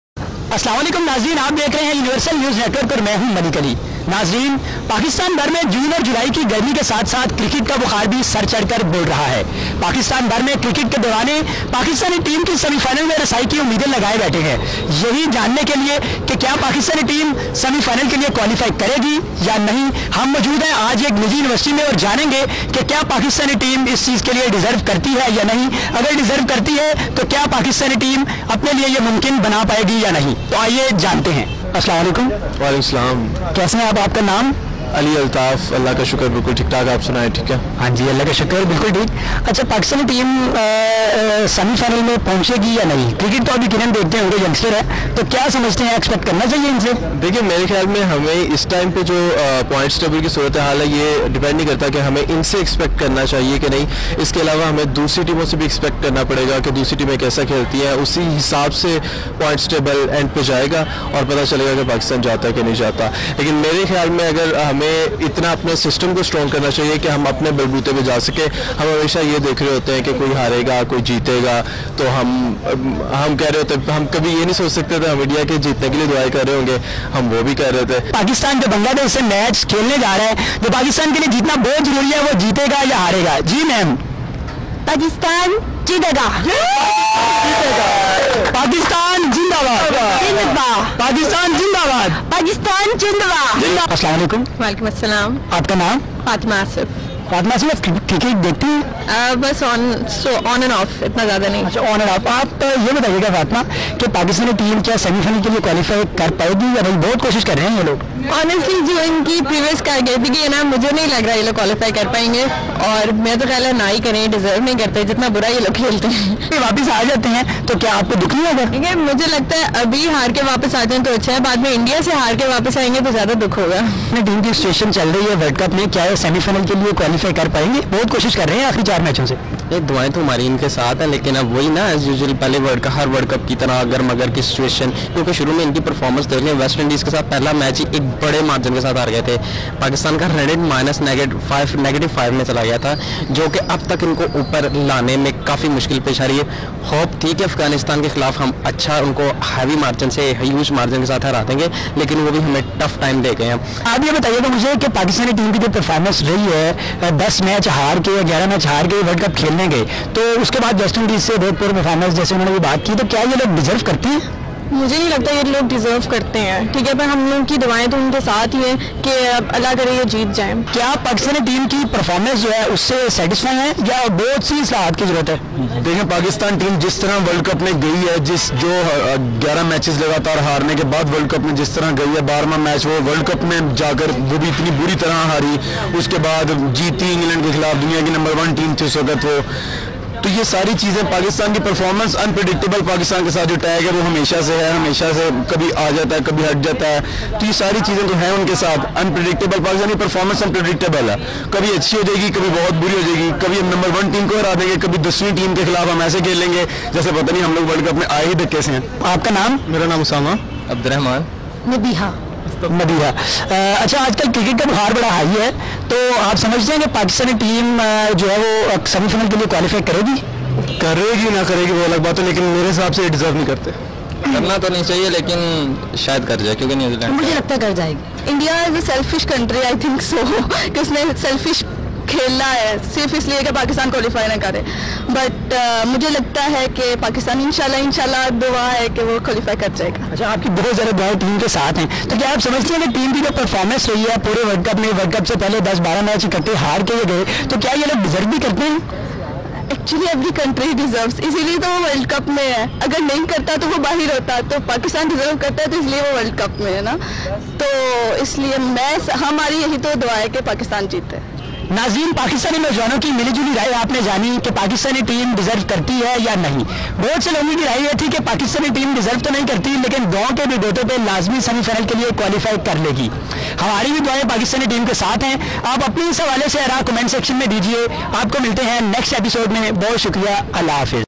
WorldCup ma pakistani team ki performance pr students ka dilchasp izhaar khayal
ورلڈ کپ میں پاکستانی ٹیم کی پر فارمنس پرسٹوڈنٹس کا دلچسپ اظہار خیال